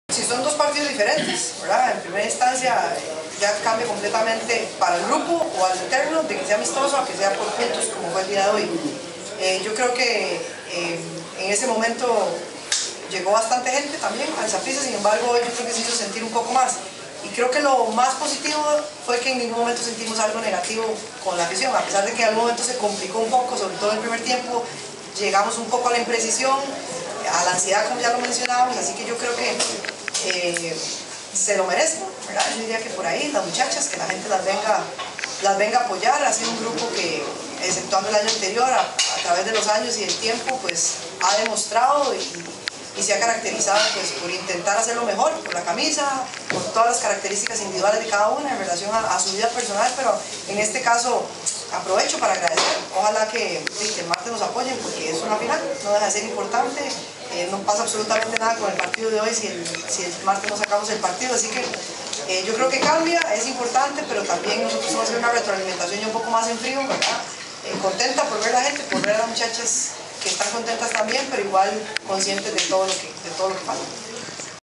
Amelia Valverde agradece apoyo a la Femenina
La seleccionadora de Costa Rica, Amelia Valverde, agradeció el apoyo que le tributó a su equipo la afición que asistió al triunfo por 2-0 ante Nicaragua, en el primer juego del Preolímpico de la Uncaf. Según la Fedefútbol, el reporte de asistencia fue de 3.200 espectadores.